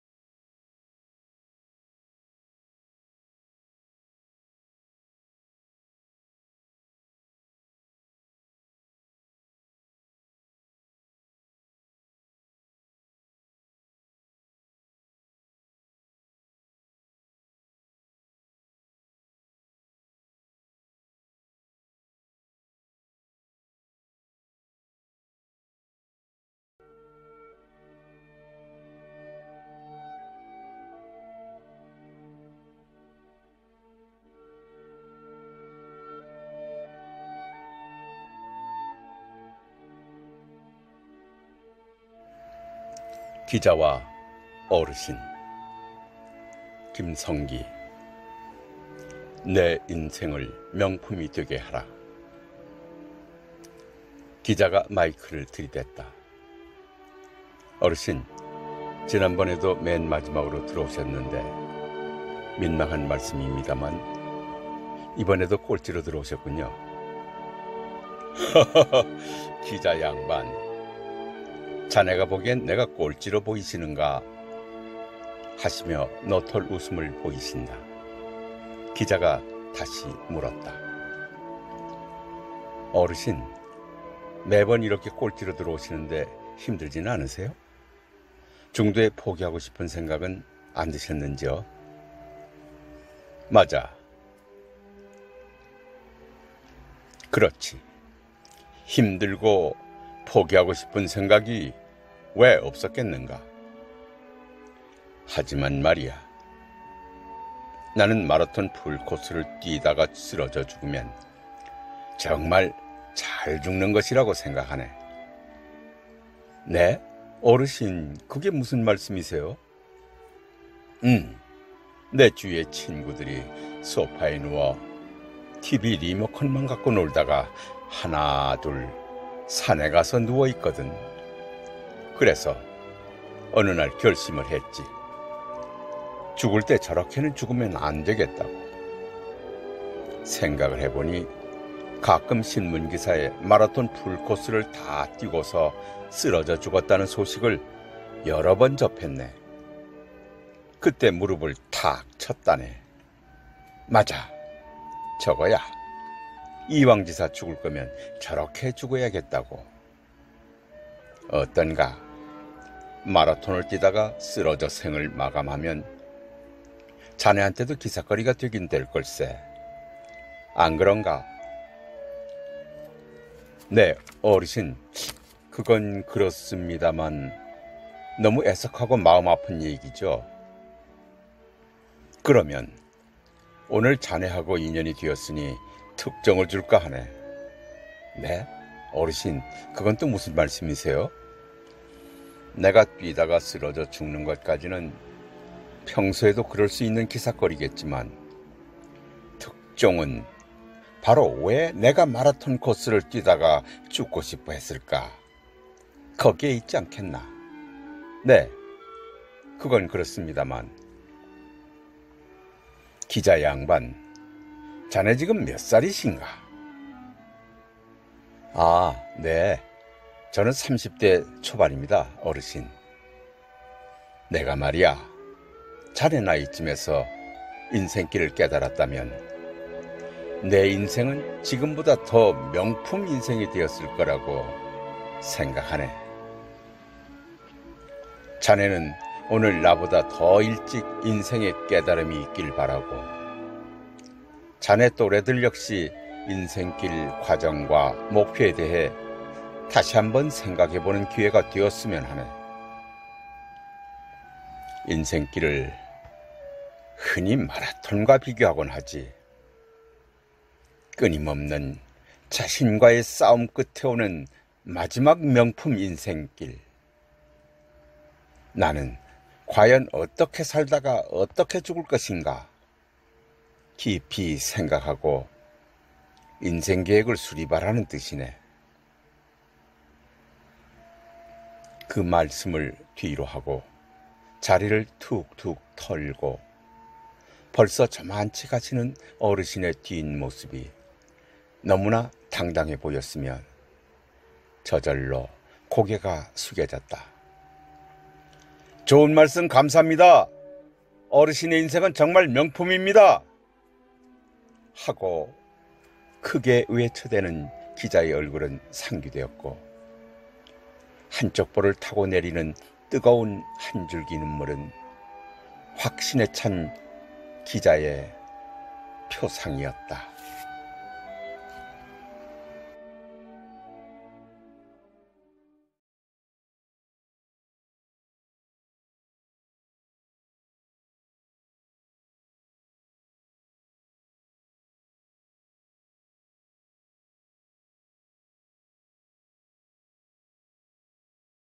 기자와 어르신(2) 내 인생이 명품이 되게하라 소나무 松木, 詩人 金成基 - 낭송 한경동 교수
문화예술 Webmagazine 송목 김성기 시인 , 수필가, 영상작가